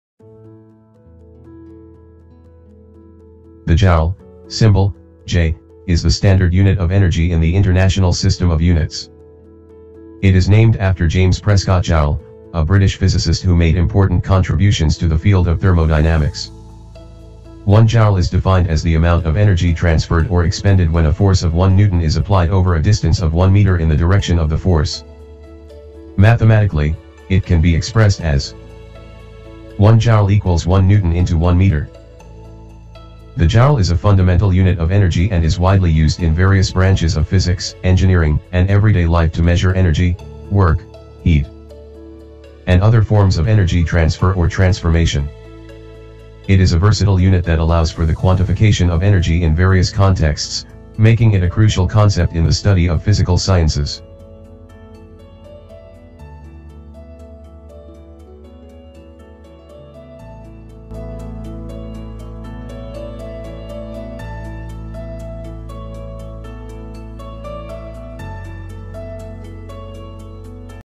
SI Unit of Work (Joule).
The joule (symbol: J) is the unit of energy in the International System of Units (SI). It is named after the English physicist James Prescott Joule, who contributed to the study of thermodynamics and the concept of energy.

The joule is defined as the amount of work done when a force of one newton is applied over a distance of one meter in the direction of the force. Mathematically, it can be expressed as:

1 J = 1 N × 1 m

In addition to being a unit of energy, the joule is also used to measure other quantities, such as work, heat, and electrical energy. For example, if a machine does 100 joules of work, it means it has transferred 100 joules of energy to an object.

Here are a few common energy values expressed in joules:

- 1 kilojoule (kJ) = 1,000 joules
- 1 megajoule (MJ) = 1,000,000 joules
- 1 gigajoule (GJ) = 1,000,000,000 joules

The joule is a versatile unit that is widely used in various fields of science, including physics, chemistry, and engineering. It provides a standardized and convenient way to quantify and compare different forms of energy and energy transfers.